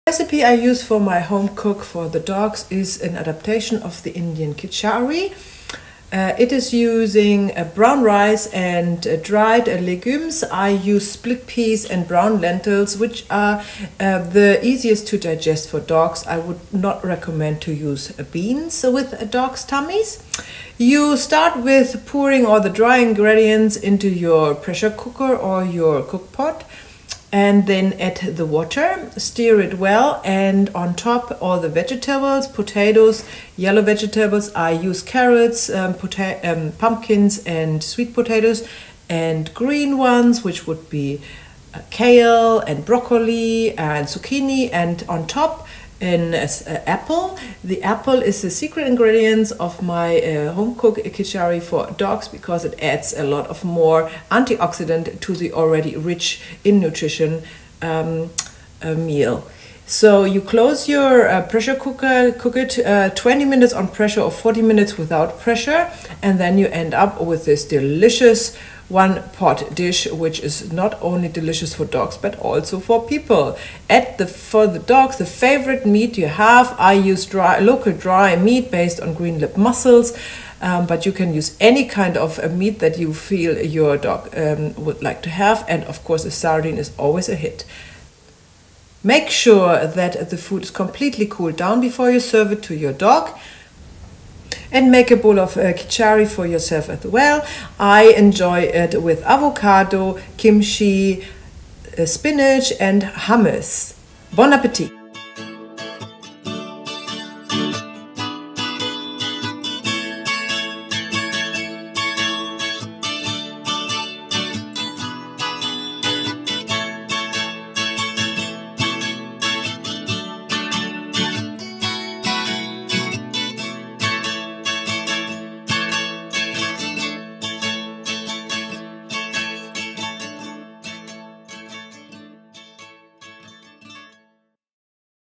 How to home-cook for your Greyhound
In this video which is part of episode 9 of our podcast show you learn how to cook with fresh and dried ingredients a nutritious and delicious every day meal for you Greyhounds and the whole family as well!